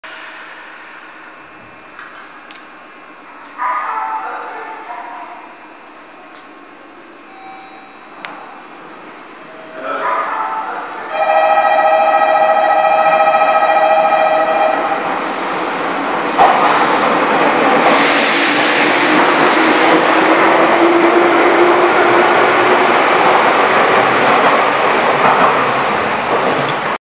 「こちらは今後どうなるのか…」6500系6503F 名鉄名古屋本線 名鉄名古屋駅 発車
※投稿する時間を調整しました。詳しい時間はチャンネルの概要をご覧ください。
2月18日撮影。
6000系が置き換えが進んでいますが6500系鉄仮面もそろそろな気がします。撮影は今からでも始めたほうがいいですね。